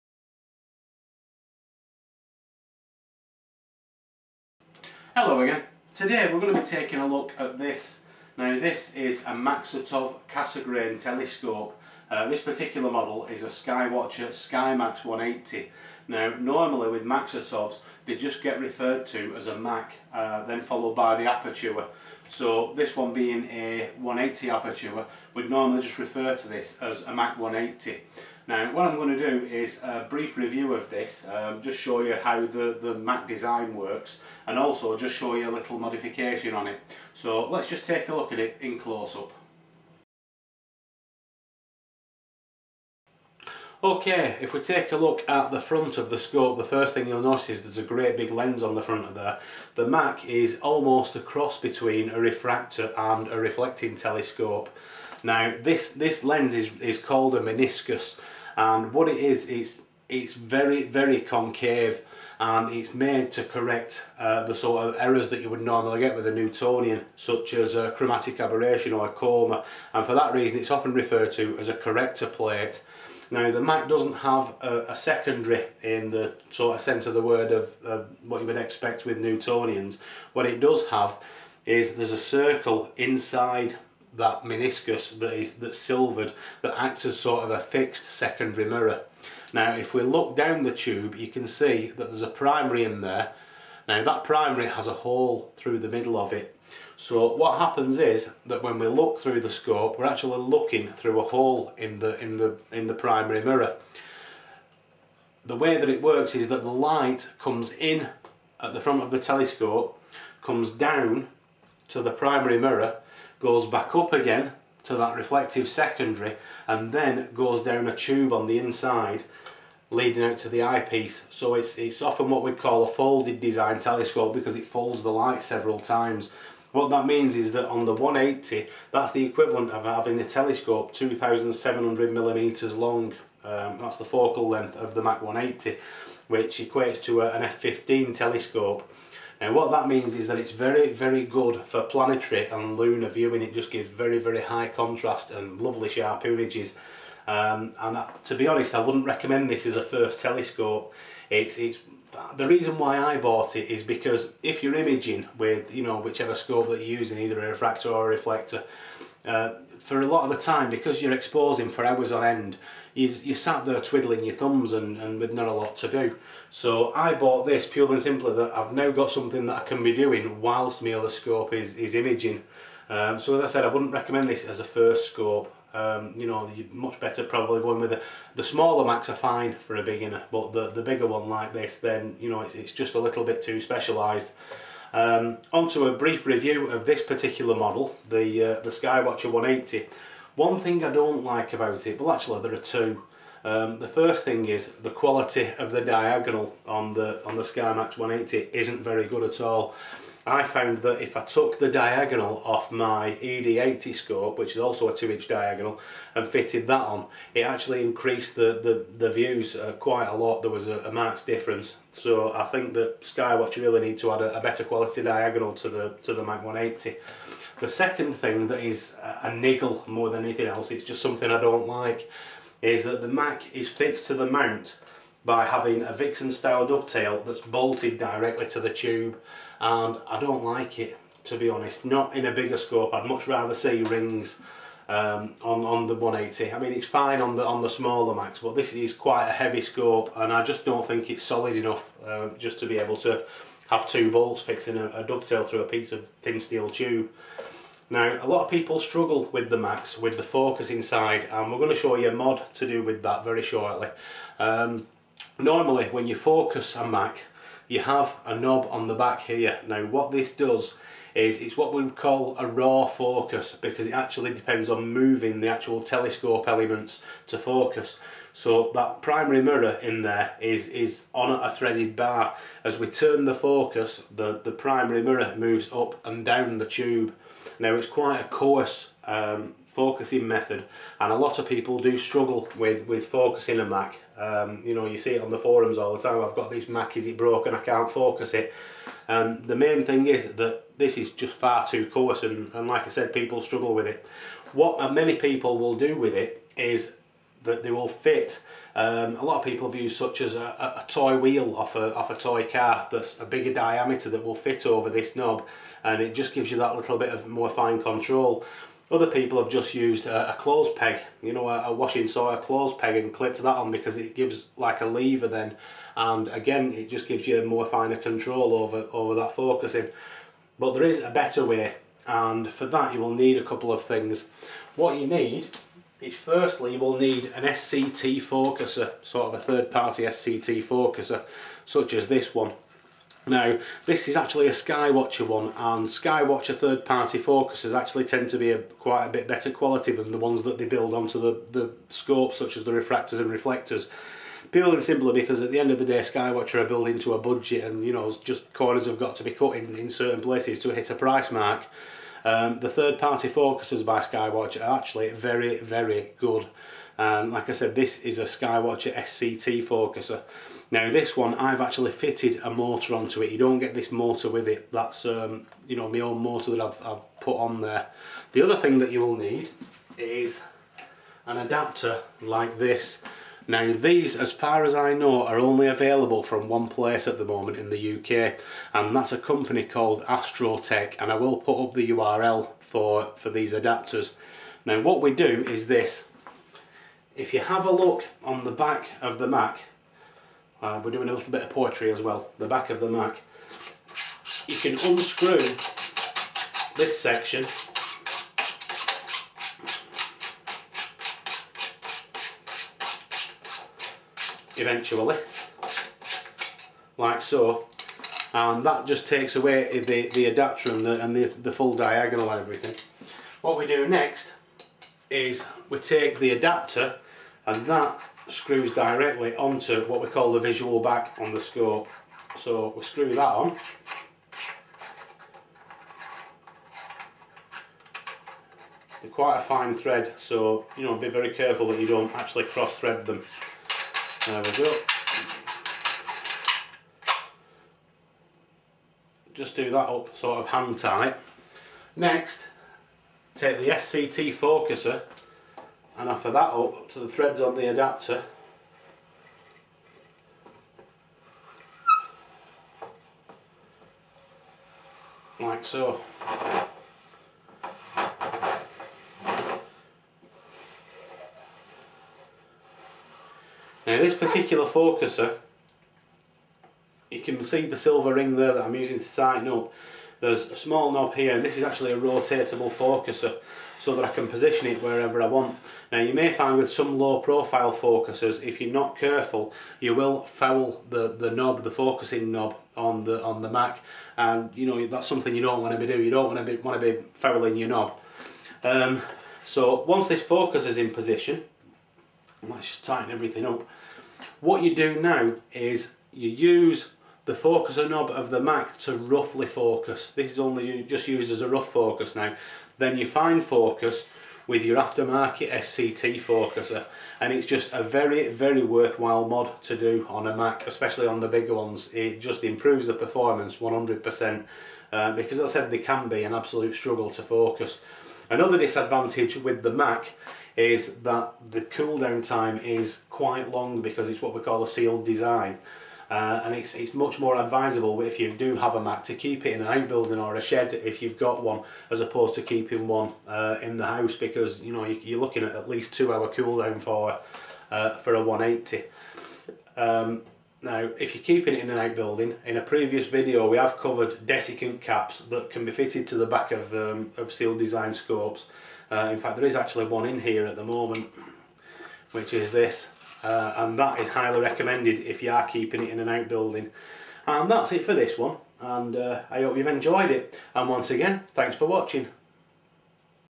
Skymax 180 Mak 180 Explanation, Review & Modding
Explanation of the Maksutov Cassegrain or 'Mak' telescope, a review of my Mak 180 and a mod.